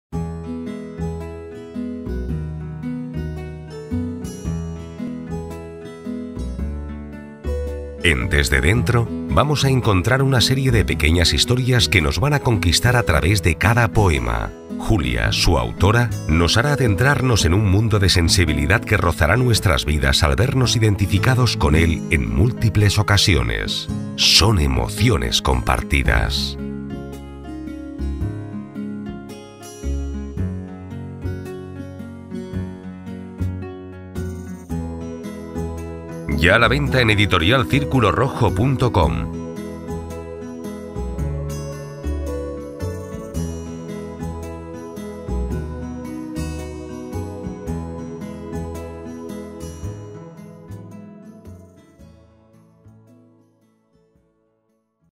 Desde dentro (Booktrailer) - Editorial Círculo Rojo
Desde dentro contiene 52 poemas de lectura fácil y emotiva.
Su poesía denota sensibilidad y frescura con un sentimiento
profundo y noble que nace en el corazón. Un soplo de ternura
que acariciará el alma del lector.